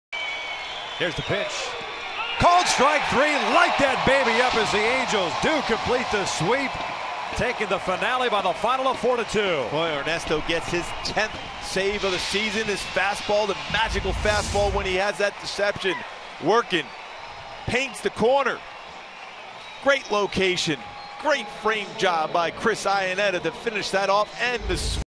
CWS@LAA: Frieri strikes out Nieto for his 10th save
6/8/14: Ernesto Frieri strikes out Adrian Nieto to record his 10th save of the season and seal both the win and sweep for the Angels